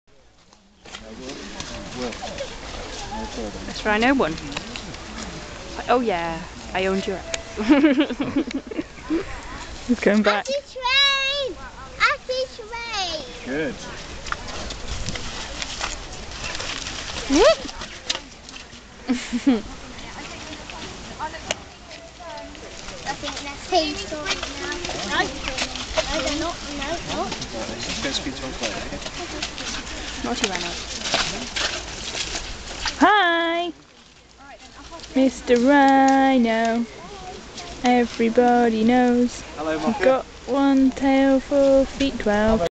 Fighting Rhinos
Fighting rhinos at Cotswold Wildlife Park, Oxfordshire on 29th June 2012 :)